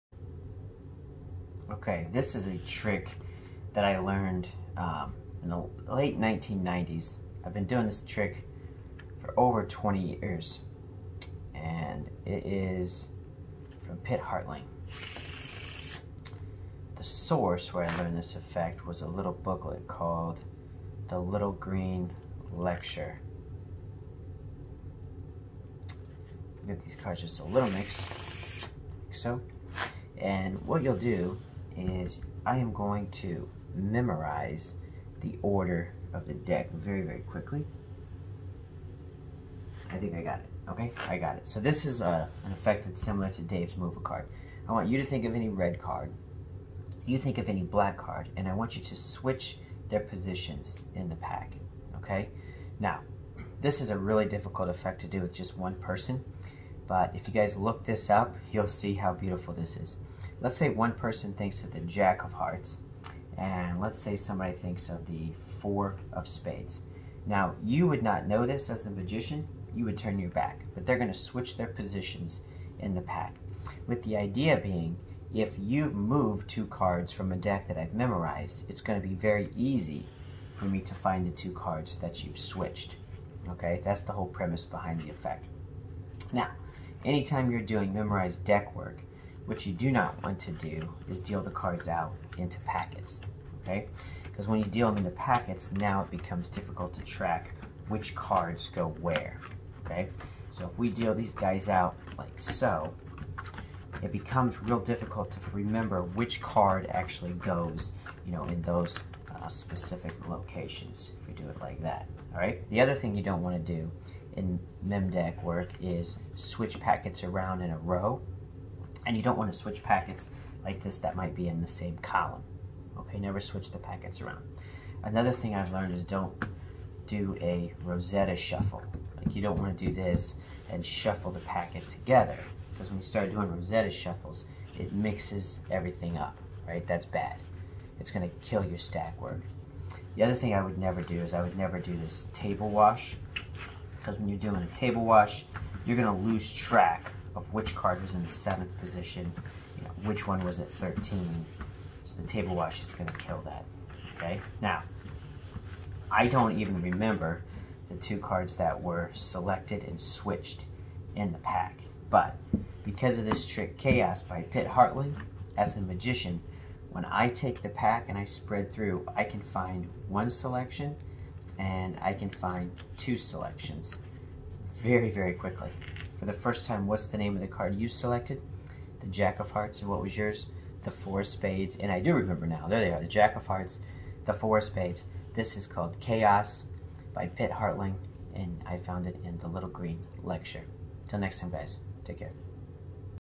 Chaos (Pit Hartling)
A very clever 2 card location using a faro shuffle, but you can set it up pre-show without the faro.